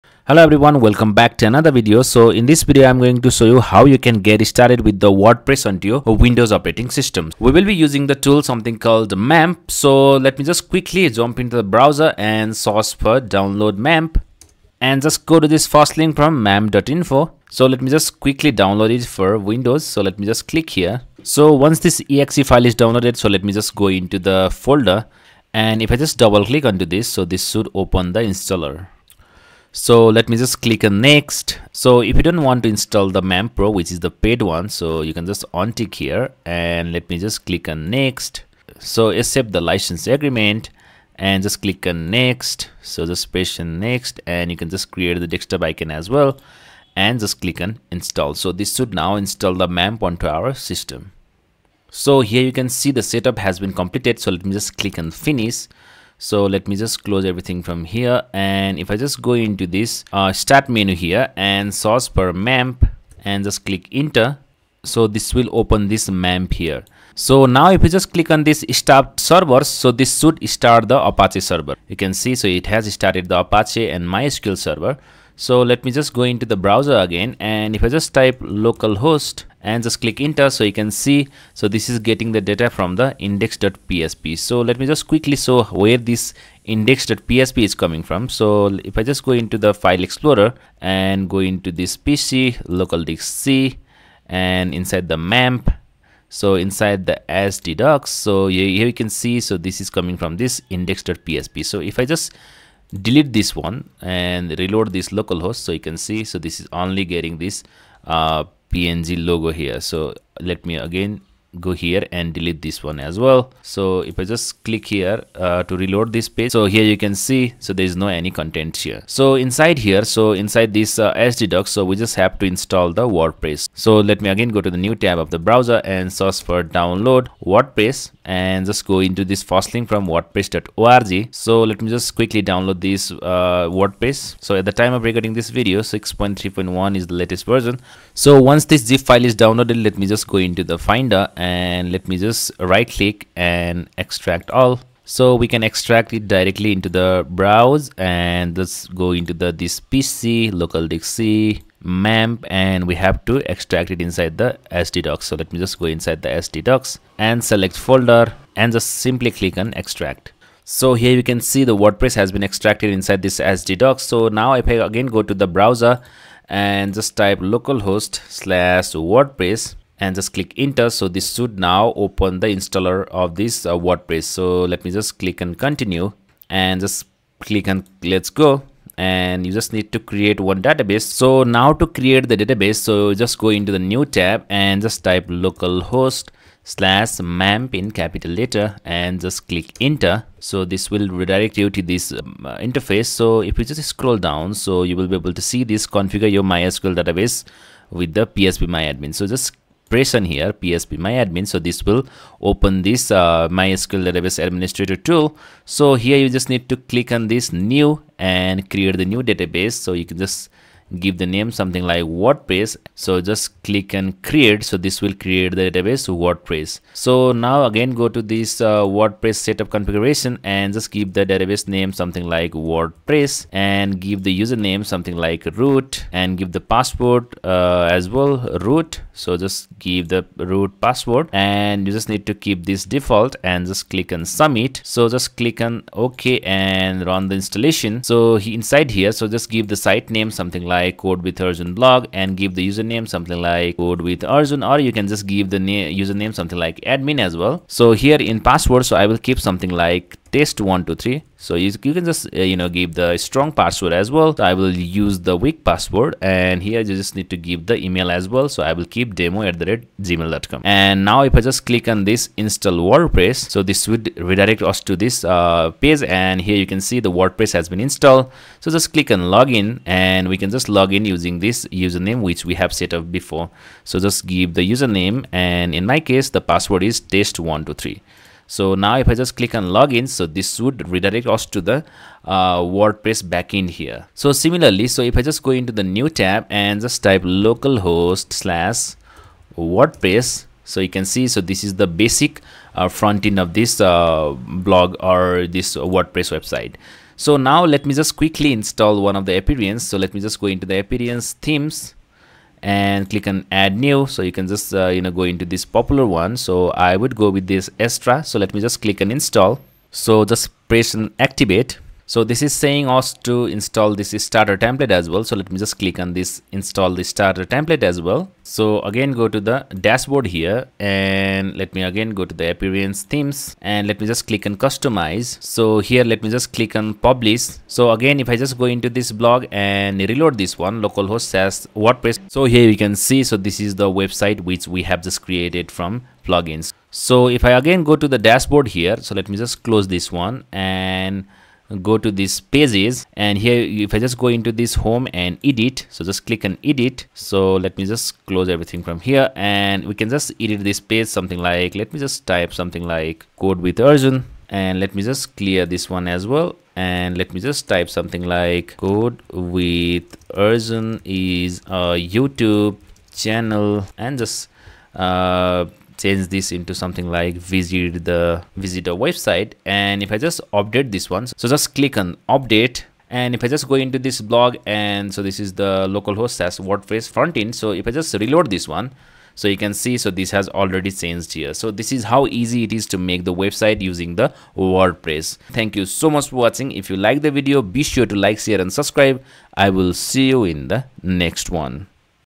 Install Wordpress on Windows using MAMP | Make website using Wordpress in a minute
n this video I have shown how you can install Wordpress on your Windows (10/11) using MAMP (Apache/phpmyadmin). I have also shown how you can create a simple website using Wordpress.

Timestamps :
00:00 - Introduction to wordpress
00:13 - Download and Install MAMP
02:06 - Download and Install wordpress
03:10 - Create database using phpmyadmin
04:50 - Change Theme using Wordpress Dashboard
05:50 - Customize Page on wordpress website
06:25 - Outro